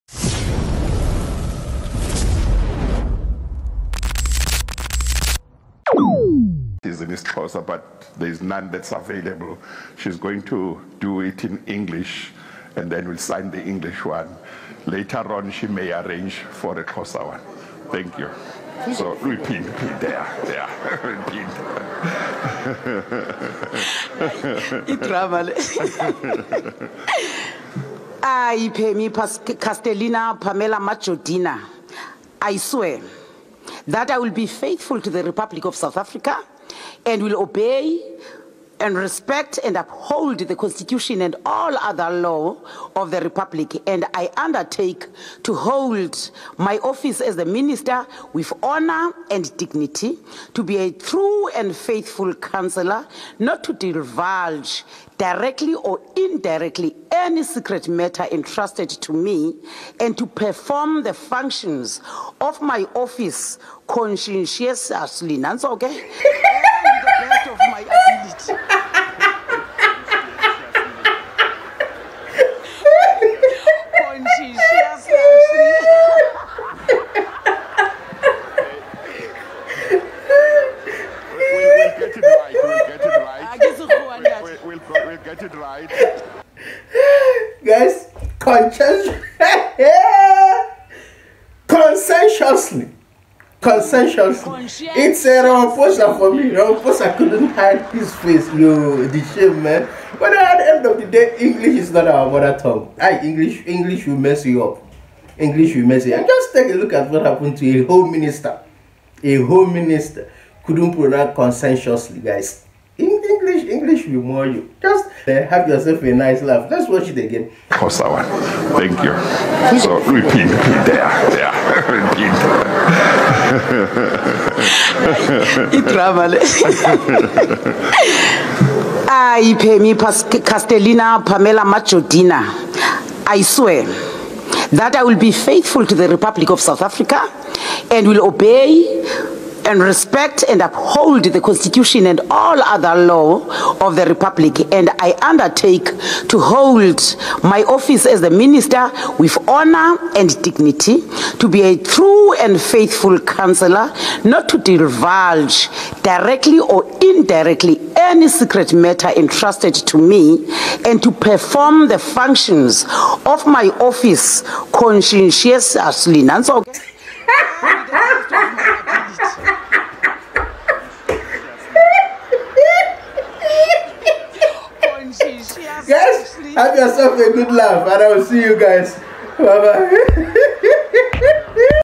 Pemmy Majodina's Hilarious Attempt at Pronouncing "Conscientiously" - Reaction Video
😂 Watch as Pemmy Majodina, the new minister, hilariously attempts to pronounce "conscientiously"! This reaction video is sure to make you laugh out loud. Join me as I react to this funny moment and share my thoughts on Pemmy Majodina's pronunciation skills.